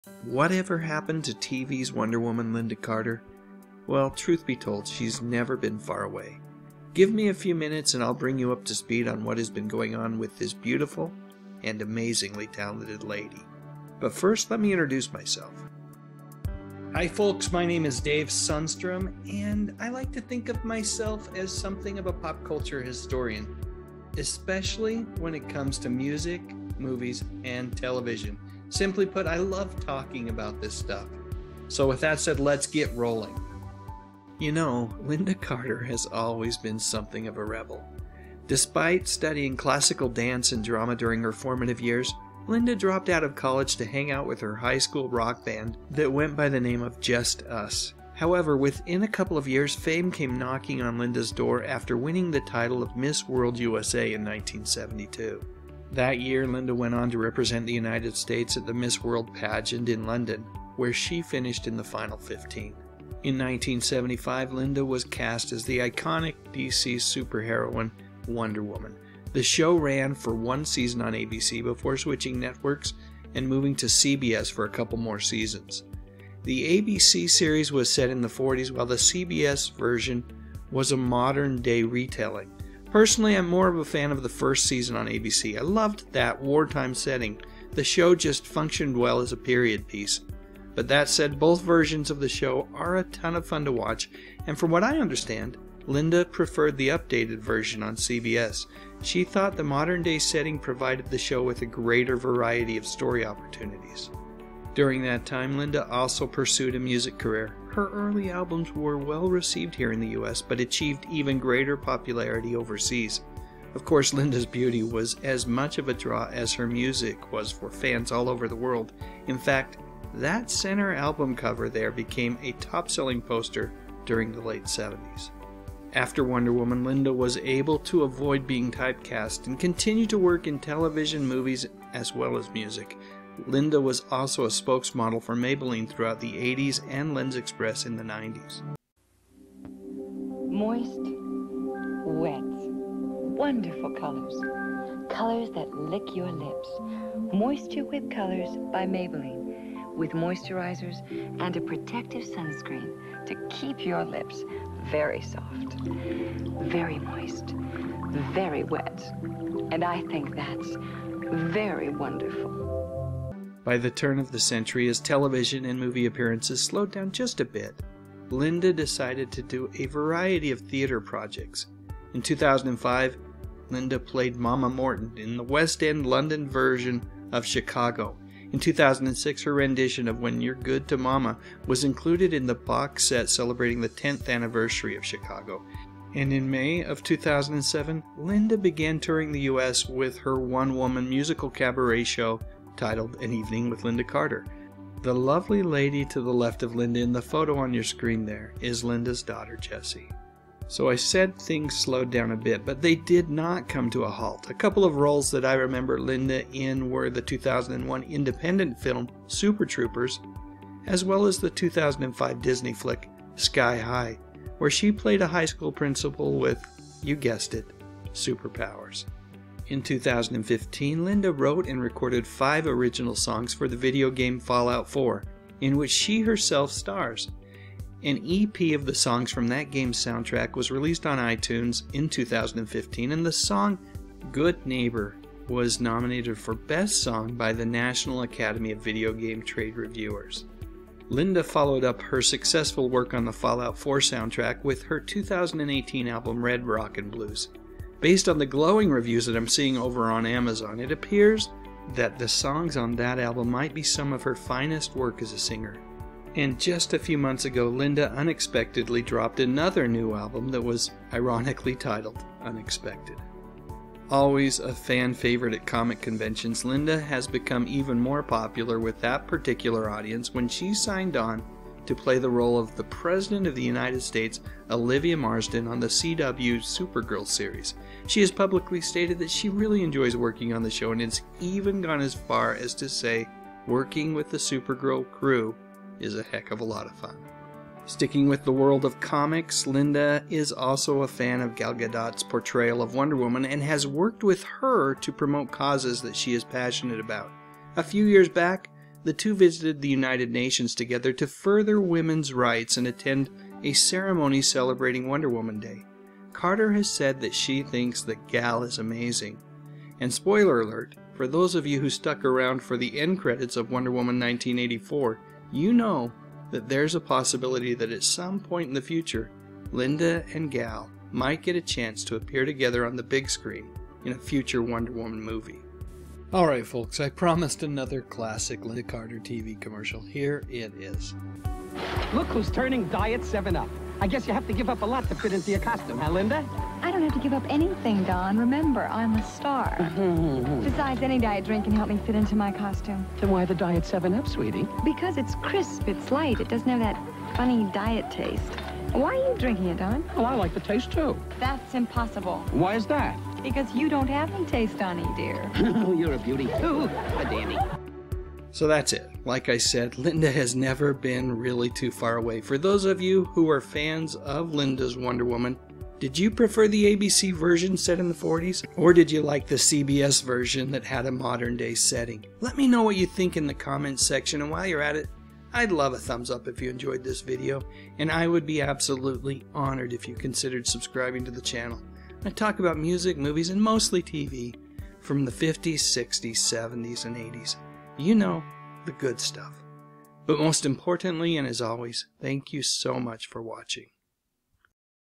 Whatever Happened to Lynda Carter - Star of TV's "Wonder Woman"
Whatever happened to TV's Wonder Woman, Lynda Carter?  Well, truth be told, she has never been too far out of the public eye.  Here's the scoop on what Lynda has been doing since hanging up her golden lasso.